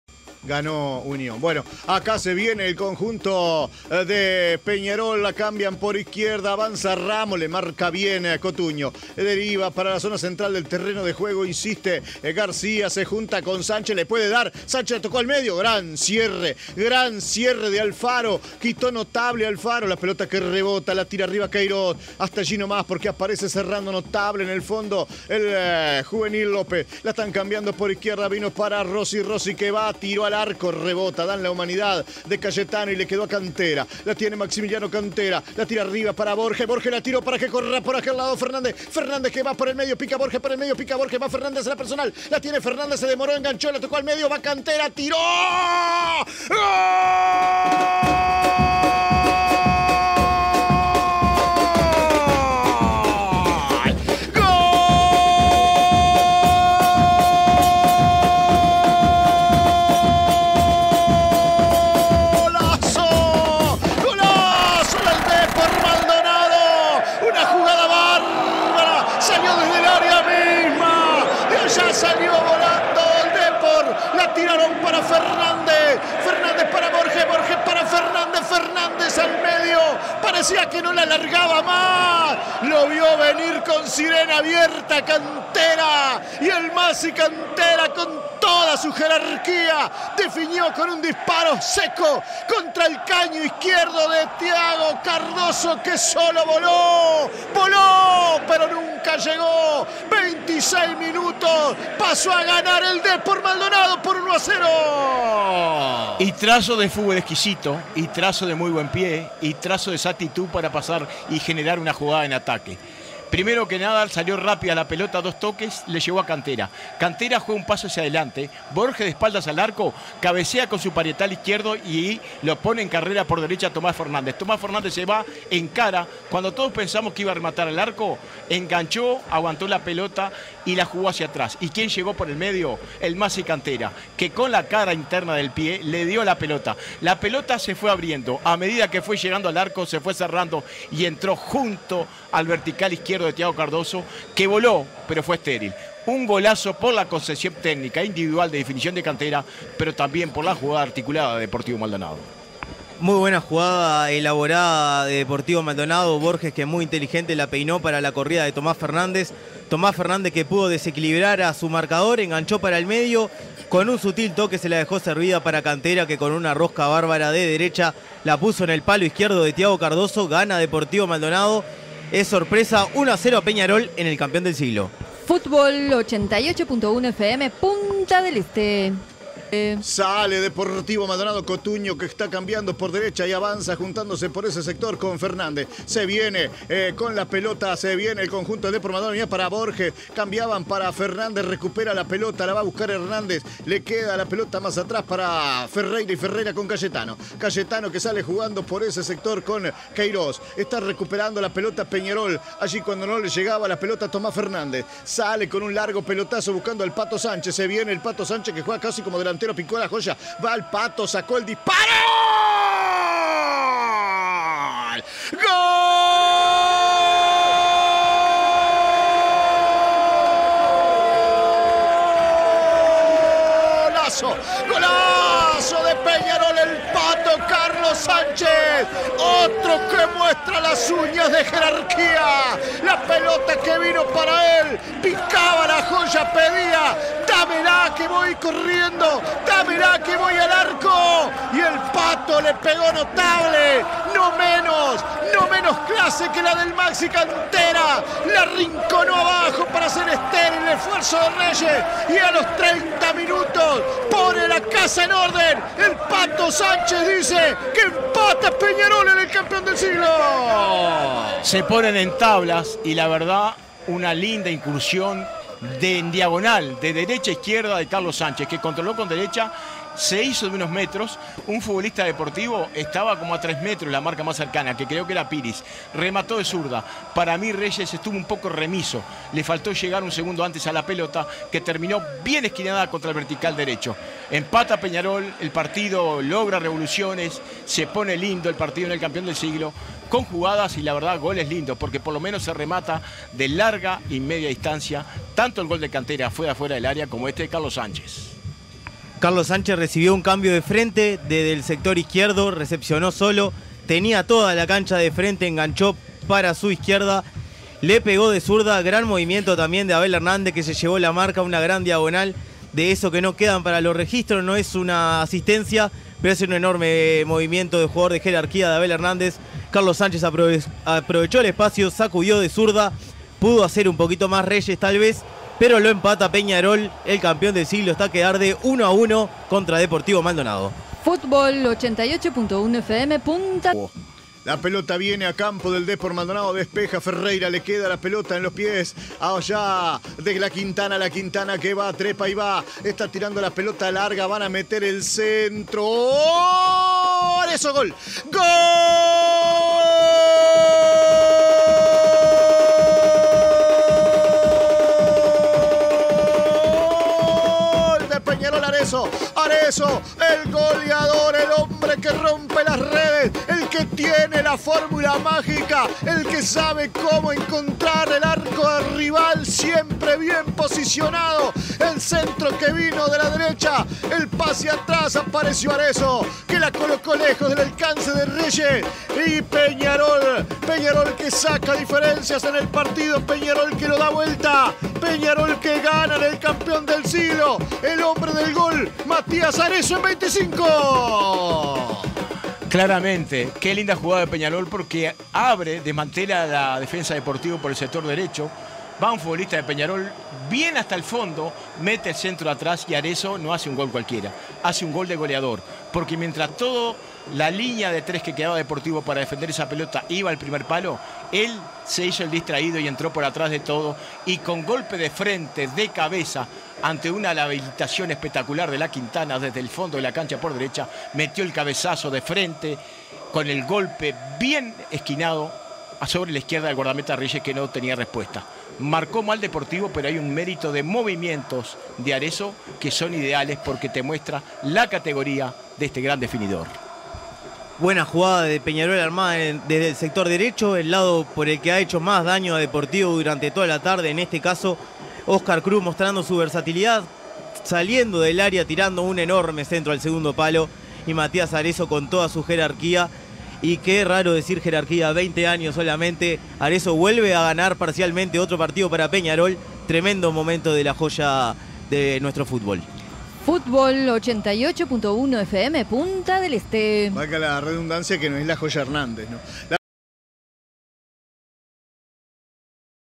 Peñarol 2 Deportivo Maldonado 1 - Apertura 2023 (por Juan Barate)
Los goles del partido, por Juan Barate junto a Leonardo AMarilla y Franco Abreu para 88.1 FM Punta del Este.